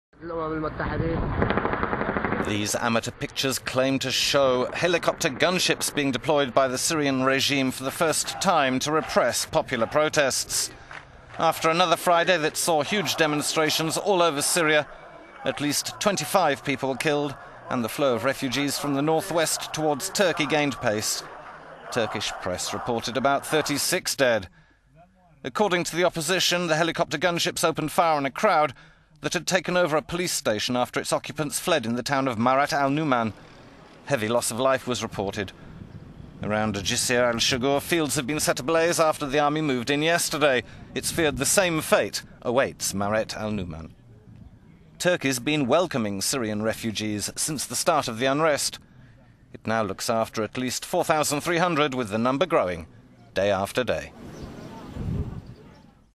Syrian helicopter gunships fire on civilians
Syria has reportedly deployed helicopter gunships for the first time to repress popular protests against the Bashar al-Assad regime. According to the opposition, they opened fire on a crowd that had taken over a police station after its occupants fled in the town of Maaret al-Nouman. Heavy loss of life was reported.

After another Friday that saw huge demonstrations all over Syria at least 25 people were killed, and the flow of refugees from the north-west towards Turkey gained pace. Turkish press reported about 36 dead.